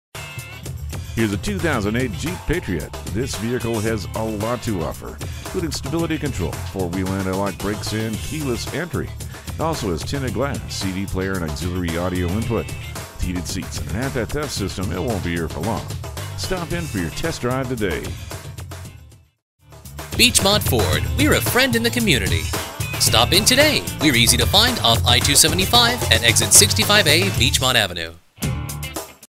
2008 Jeep Patriot Cincinnati Dayton, OH PT-17034 - SOLD
Year: 2008
Make: Jeep
Model: Patriot
Trim: Sport
Engine: 4 Cyl. 2.0L
Transmission: Manual
Color: Jeep Green Metallic
Interior: Pastel Pebble Beige
Mileage: 86686
Stock #: PT-17034
HEATED FRONT SEATS, MP3 CD PLAYER, AND CRUISE CONTROL. This 2008 Jeep Patriot FWD 4dr Sport is value priced to sell quickly! It has a great looking Jeep Green Metallic exterior and a Pastel Pebble Beige interior! Please call us to confirm availability and to schedule a hassle free test drive. We are located at: 600 Ohio Pike, Cincinnati, OH, 45245.

Address:
600 Ohio Pike